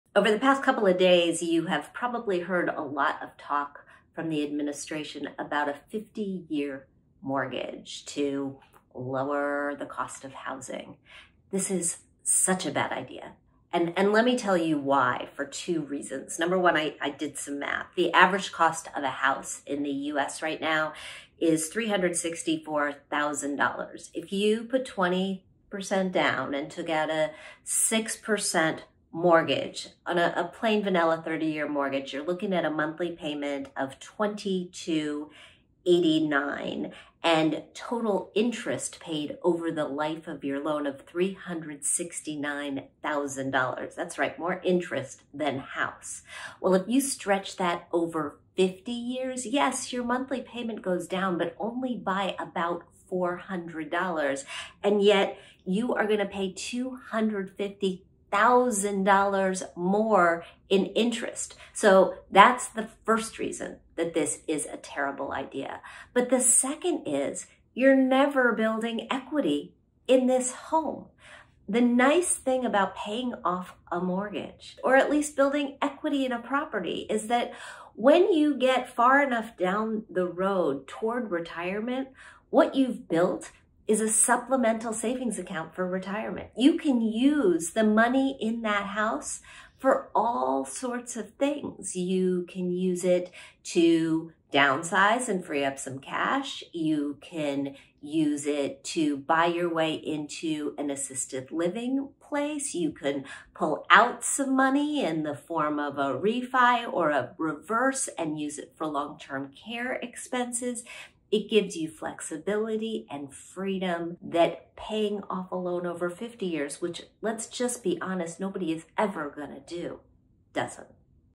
I did the math on 50 year mortgages. It's bad.
You've probably heard talk about 50-year mortgages to help with housing costs. Here's why it's a terrible idea — and the math that proves it.